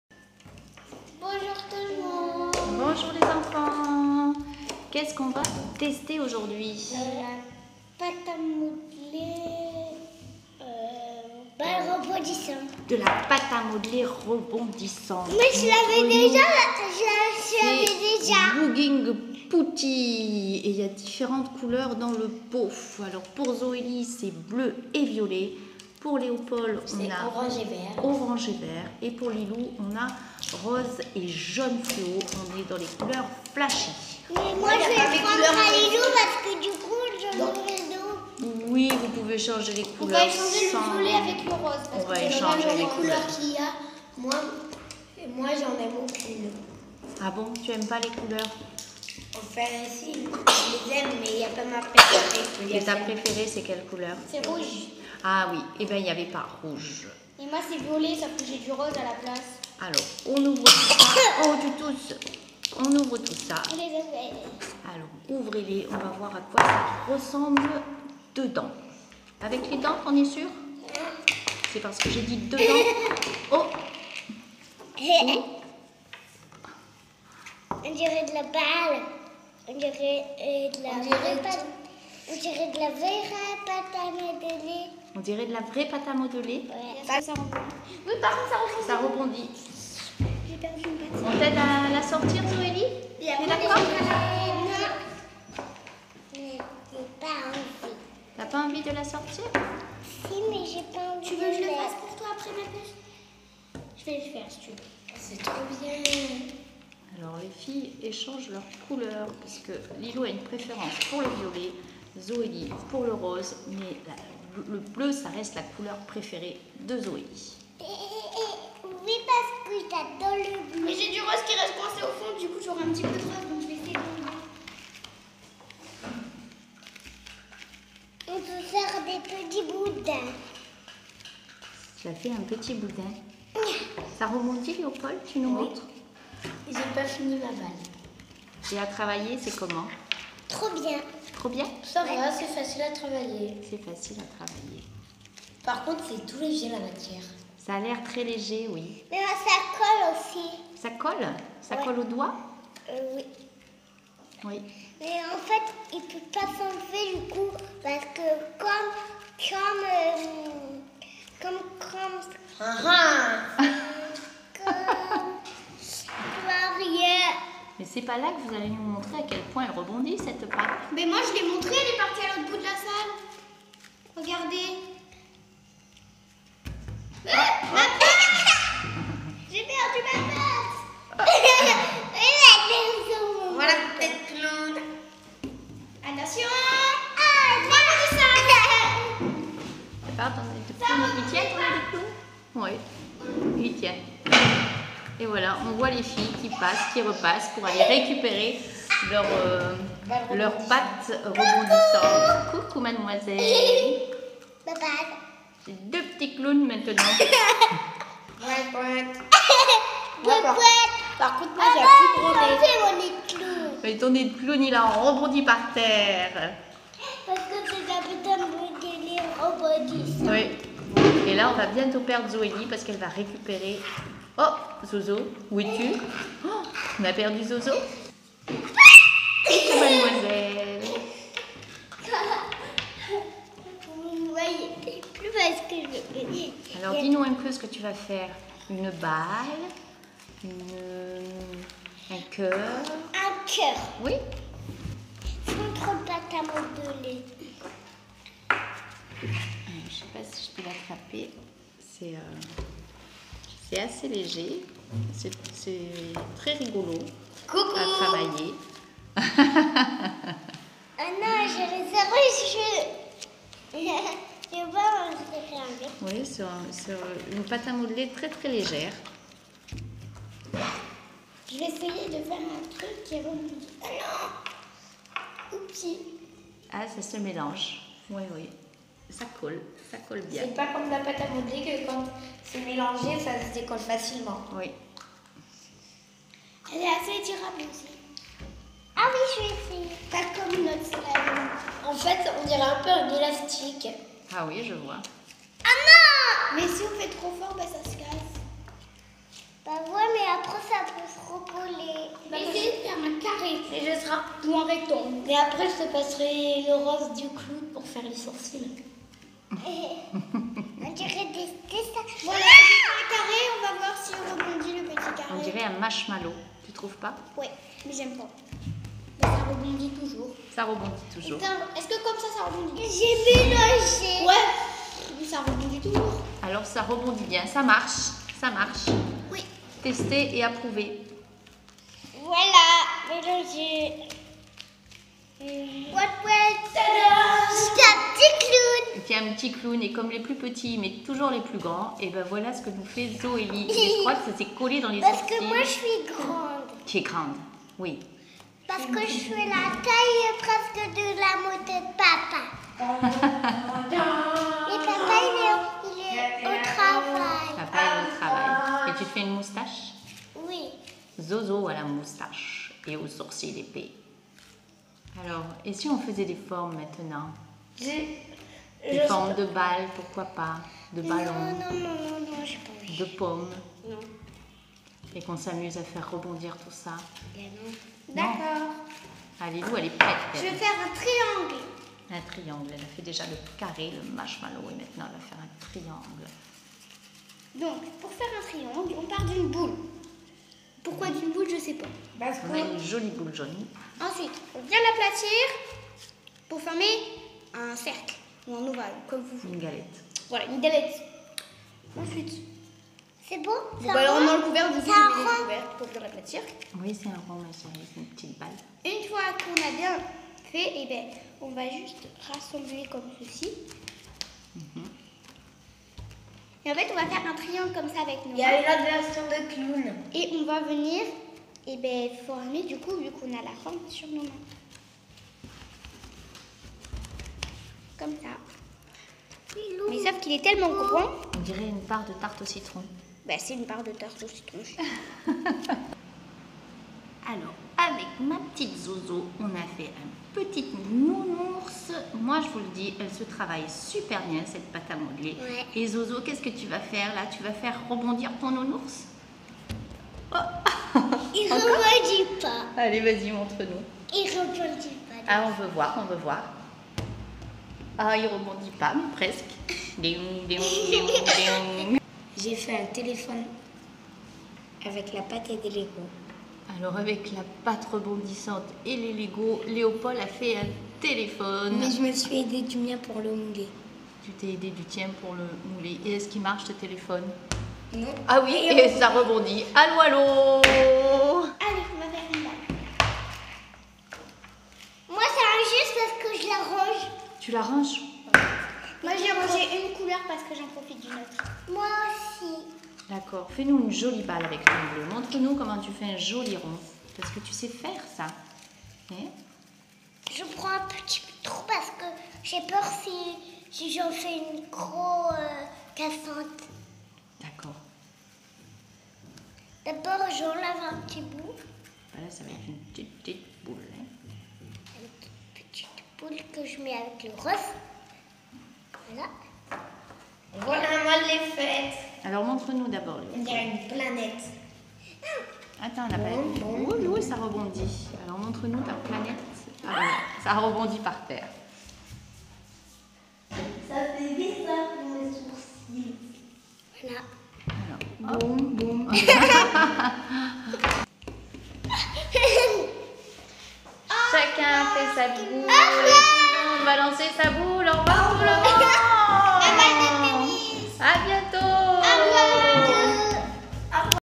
TRIO-LO, bouncing putty
Zoé-Lee, Léo-Paul et Lee-Lou testent de la pâte à modeler rebondissante, "BOUNCING PUTTY", très très sympa. deux couleurs dans un pot et chacun à fabriquer sa balle